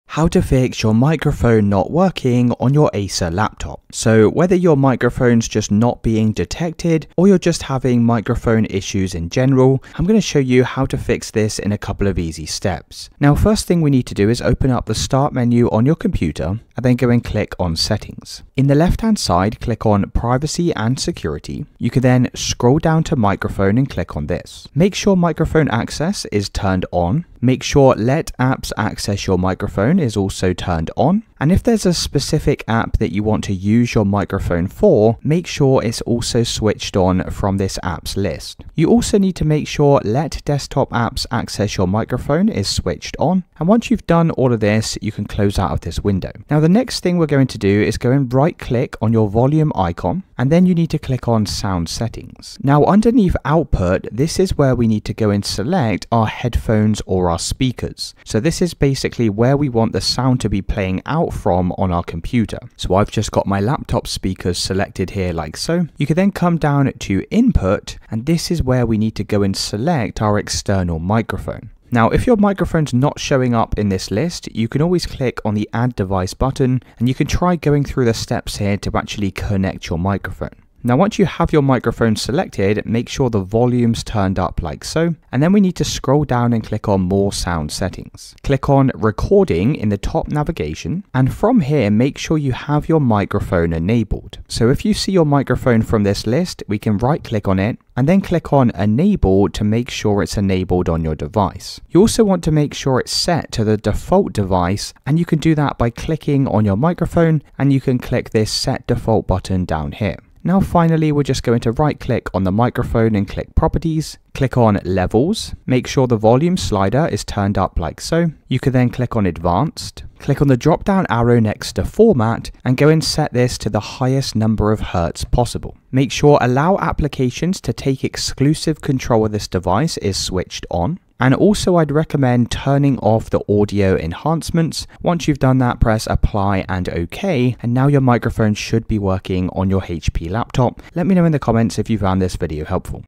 How To Fix Microphone Not Working On Acer Laptop
Fix an Acer laptop microphone that isn’t working by checking input settings and adjusting privacy permissions. This guide covers mic issues, external microphone selection, and common Windows 11 audio input problems.